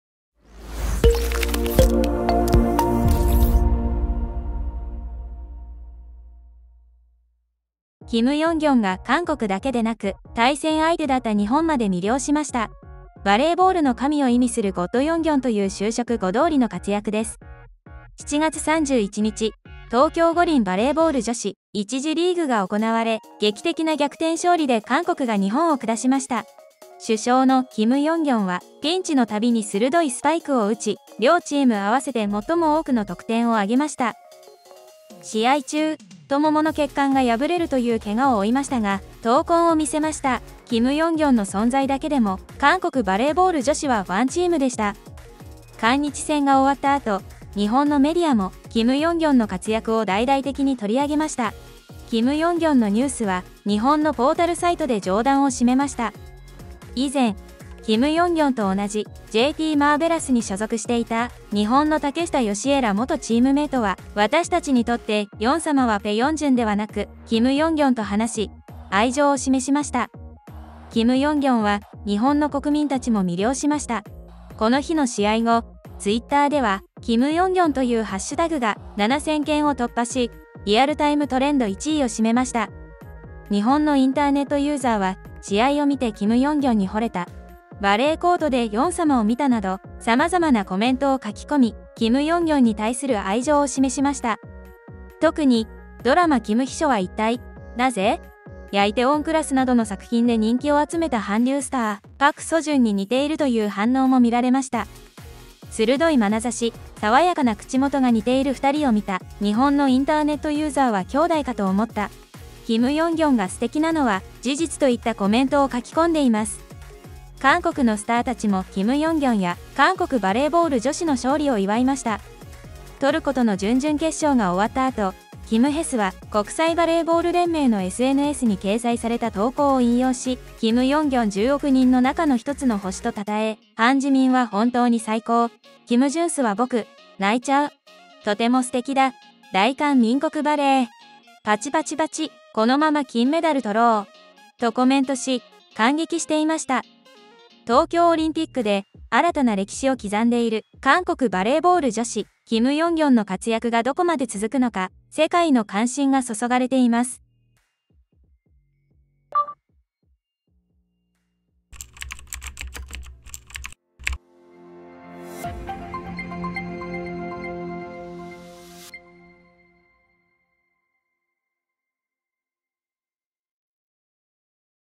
『梨泰院クラス』パク・ソジュンにそっくり？ 日本を魅了した「金軟景クラス」
金軟景（キム・ヨンギョン）が韓国だけでなく、相手チームだった日本まで魅了した。バレーボールの神を意味する「ゴッド・ヨンギョン」という修飾語どおりの活躍だ。